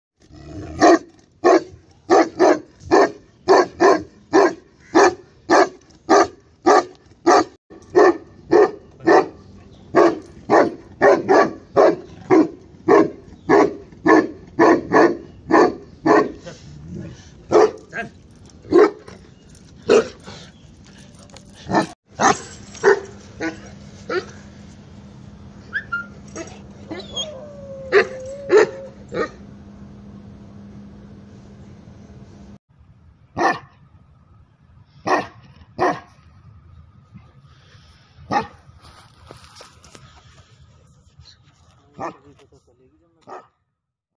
Gaddi puppy soled last year (last sale gaddi pupps look like cham )
Gaddi puppy (Iss video main hum apko dikha rahe hai Hamare gaddi dogs ki quality) hum apko denge original Himachali Gaddi puppy Gaddi Dog  gaddi puppy for sale